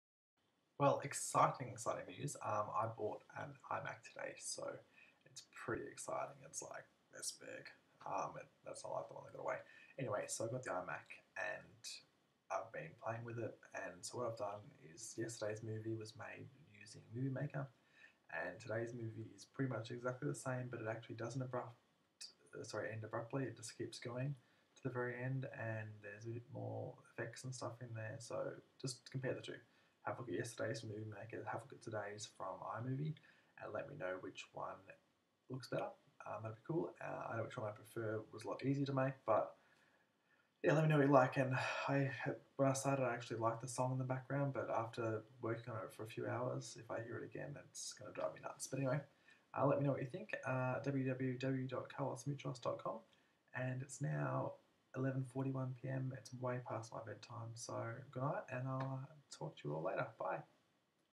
Video Blog - I got an iMac
Well despite heaps of people telling me not to go to the dark side and buy an apple computer, today I lashed out and got myself a sexy 27" iMac, which is where today's Video Blog is coming to you from. Why the purchase? Well partially because I really want to make some videos, and the PC just doesn't cut it, and also because its white, its clean and it looks great. Sure I'm yet to really work out how the mouse works, or how to click stuff, but I'm getting there. I have spent the last few hours playing around with iMovie to try and mirror what I attempted in Movie Maker the day before. So in today's Video Blog you actually are getting two Videos. One of me being excited about my new purchase, and the other showing off what I whipped up in a couple of hours. Let me know which of the two movies you prefer, the PC or iMac version.